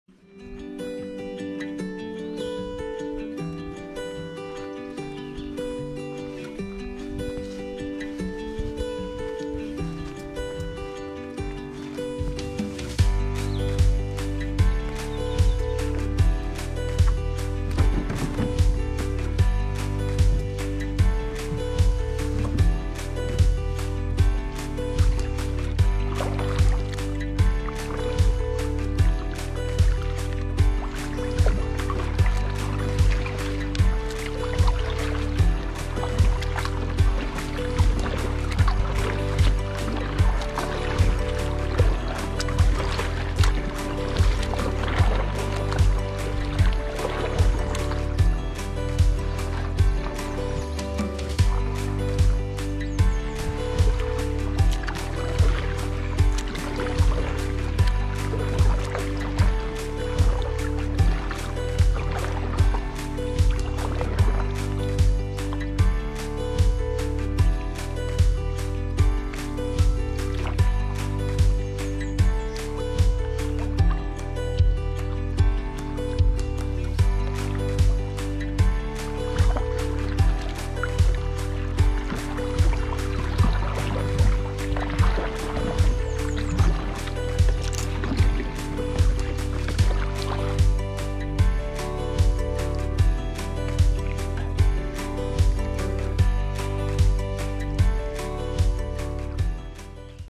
Paddling the Willis River in Cumberland State Forest, Virginia
Video shot during a recent paddling trip on the Willis River in the Cumberland State Forest in Virginia.  One of us was in a Jackson Bite kayak, the other a Field and Stream Eagle Talon kayak.  The outing had some good and bad elements.  Good: beautiful scenery on a wilderness river with no development or farmland on either bank, just woods--a rare thing on the east coast.  Bad: lots of swarming bugs (this was in April) that bug spray had no effect on, and also many fallen logs/trees either blocking or partially blocking the river.  Bottom line, if you want to travel more than 1/2 mile in either direction on the river, be prepared to lift your boat over some fallen trees.